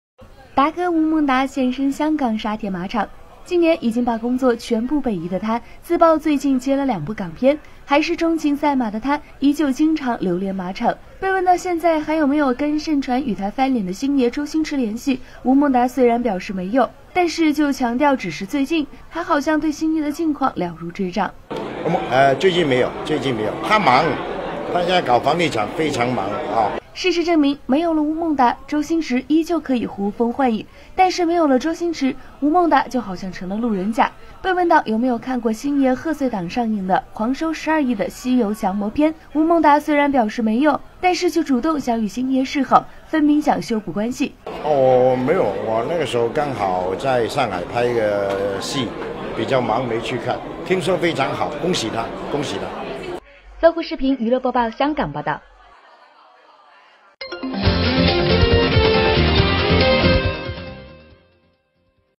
《娱乐资讯》吴孟达主动示好周星驰 疑想修补关系
达哥吴孟达现身香港沙田马场，近年已经把工作全部北移的他自曝最近接了两部港片，还是钟情赛马的他依旧经常流连马场，被问到现在还有没有跟盛传与他翻脸的星爷周星驰联系吴孟达虽然表示没有，但是就强调只是最近还好像对星爷的近况了如指掌